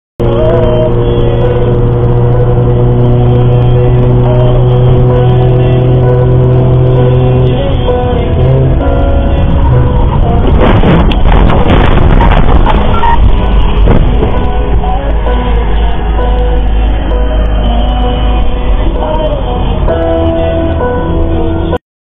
Dash cam videos compilation 2021. Caught on camera. dvr №7
Compilation of recordings from DVRs. In these videos you will see many different cases that can happen along the way. The road is fraught with many different troubles and to capture them you need a DVR. Many have already got video registrars for the car and are shooting interesting videos. Let's watch some interesting videos, there's a lot to see!